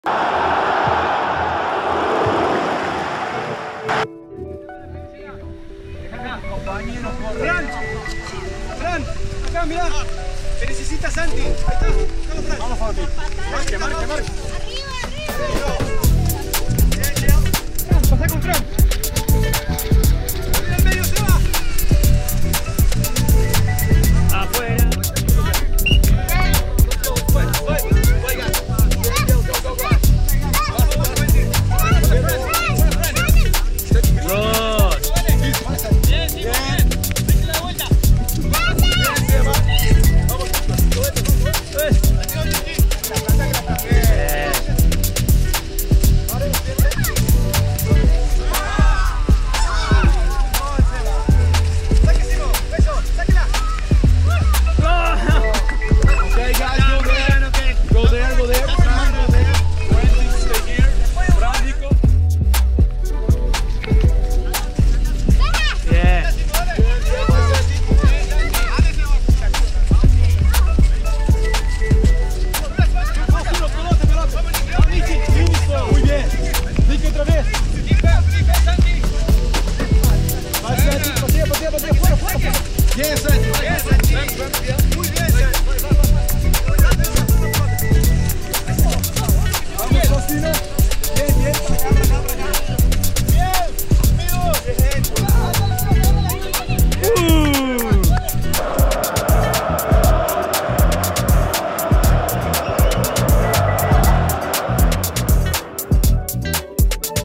TOCATV: MINIS Training Program
⚽️ MINIS soccer
☔️ Nature can’t stop the MINIS
Great to see them come out even in the rain  🌧